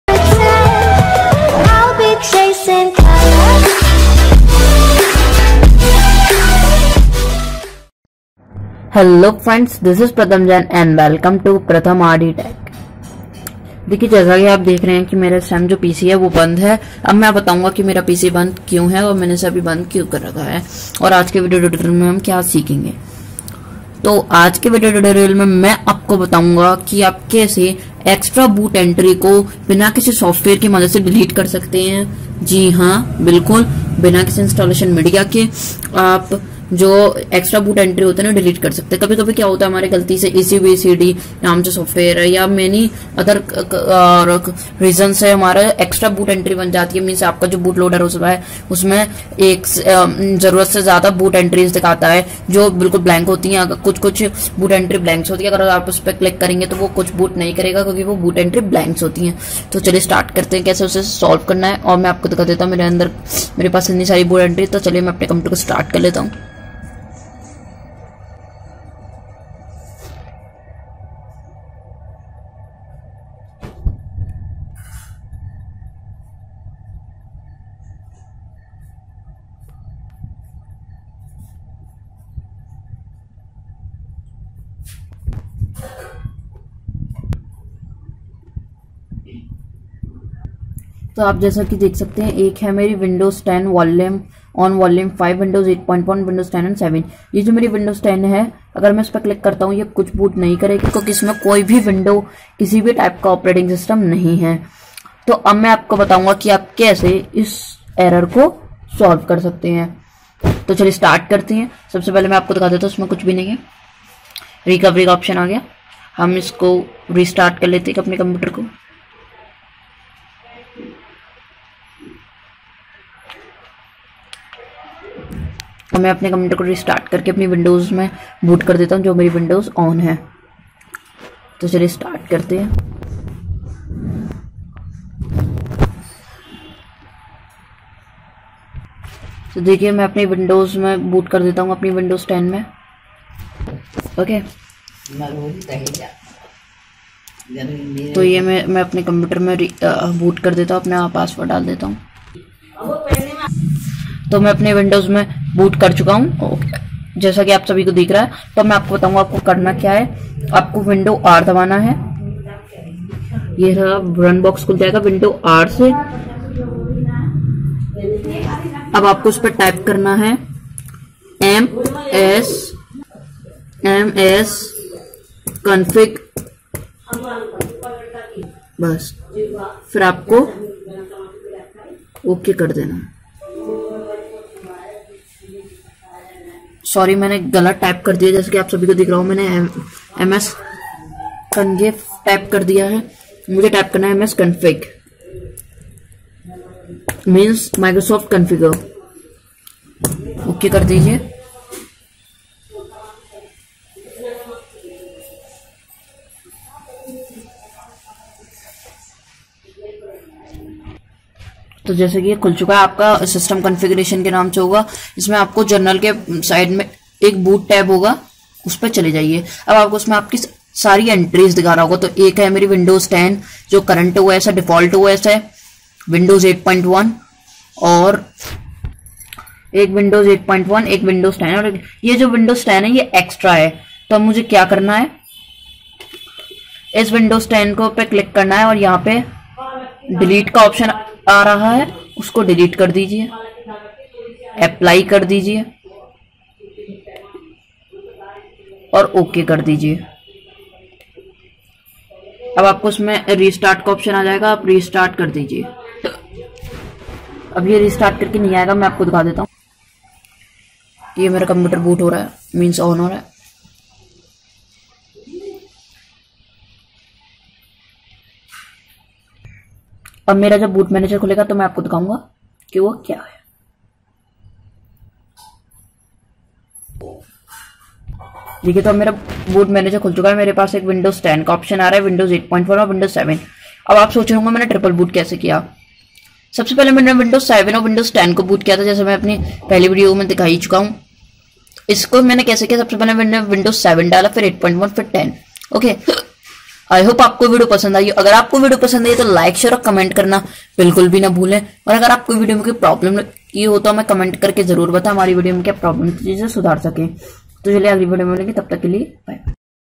How to remove extra BOOT ENTRY without any software.
Hello friends in this video I will show you how to remove extra boot entry without any software.